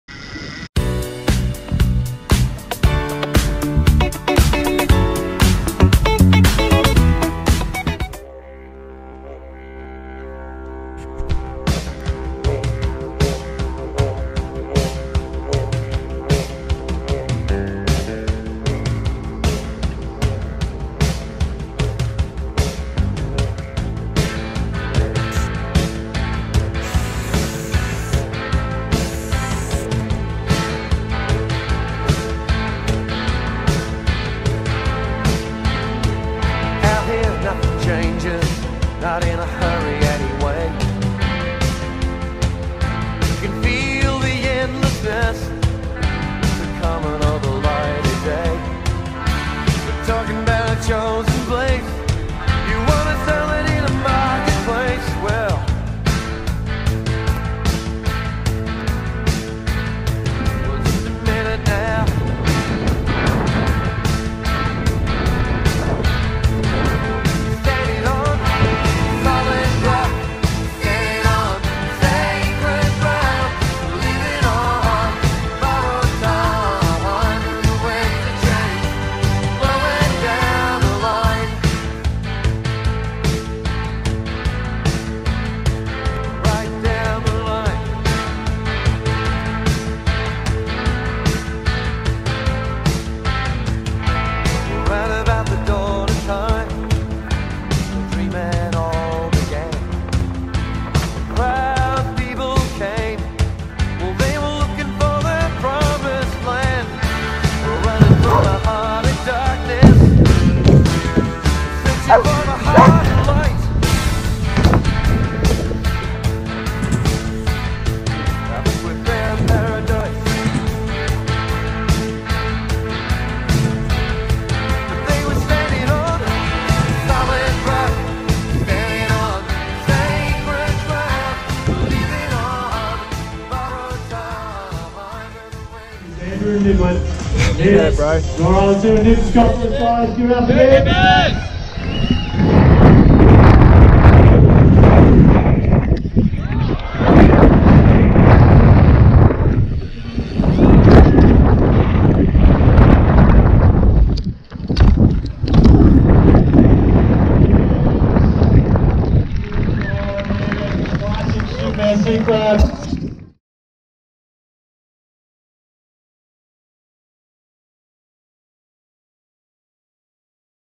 NEW BIKE AND NEW RESI MAT! Greenbike Webisode #42
Unboxing and building up a sweet new steel hardtail dirt jumper. And after 5 years and 1000s of jumps on the resi mat it was time for an upgrade. Also in this vid a run down the Highline Slopestyle course at Mt.Buller.
Filmed on GoPro 6.
Edited on iMovie.